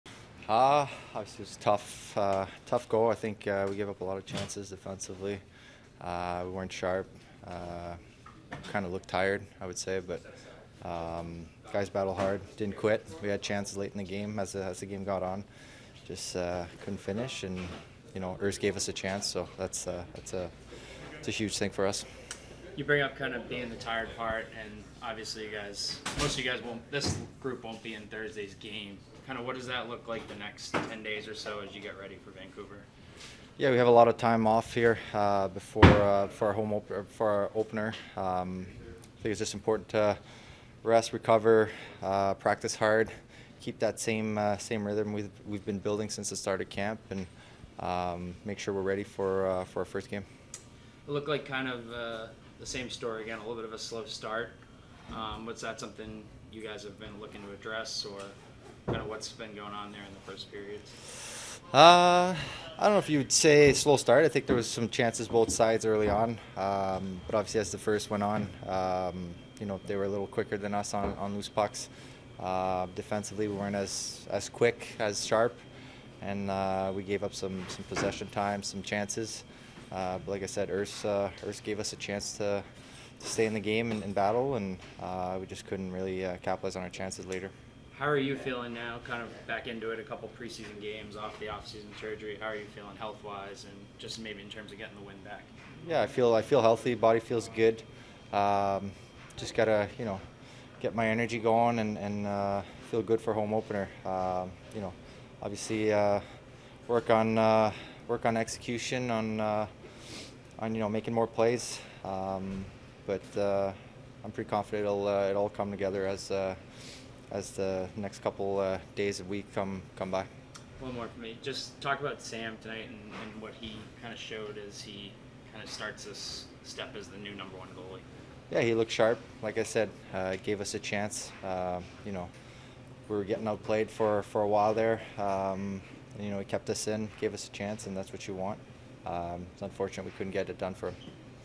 10/1 PHI vs. BOS Postgame: Sean Couturier
Hear from Sean Couturier following a 4-1 loss to the Bruins.